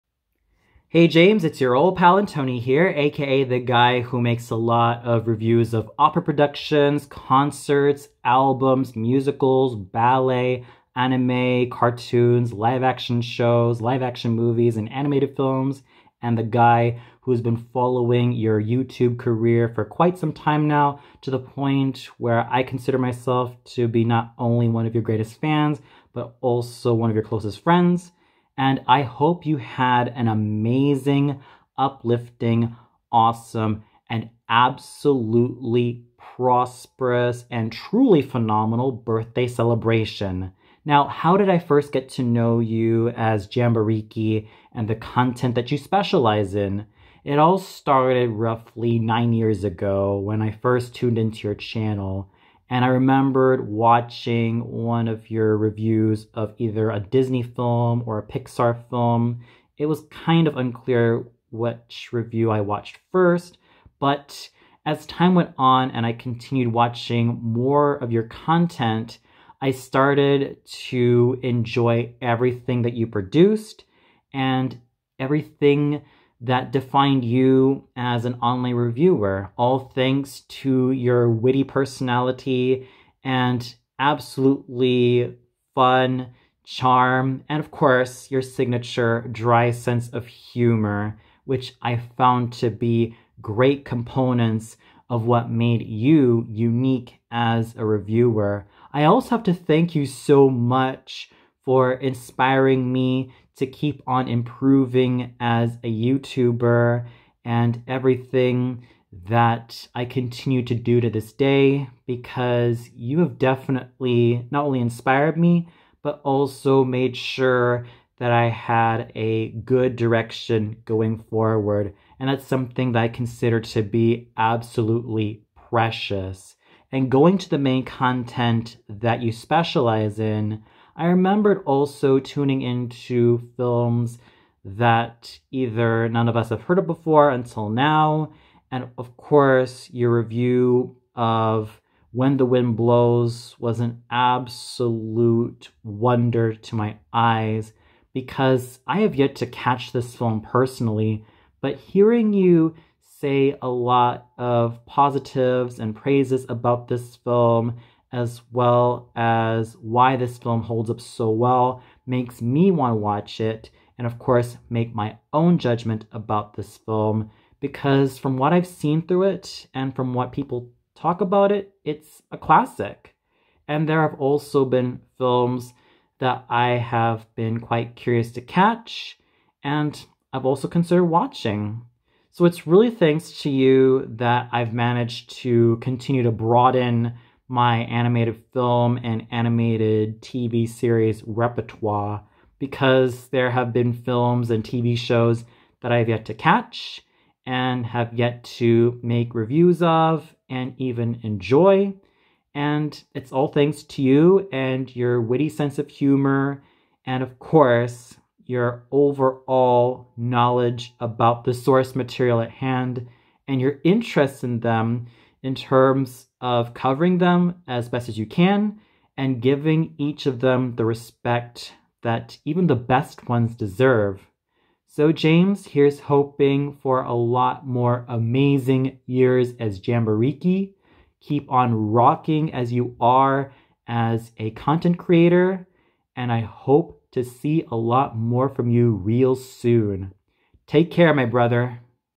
My Belated Birthday Shout-Out to James McLoughlin aka Jambareeqi
Here's to more years of prosperity and awesomeness to the awesomely witty James McLoughlin aka @Jambareeqi. I hope you had an amazing birthday, and continue to be the direct and charismatic content creator that you are, my brother.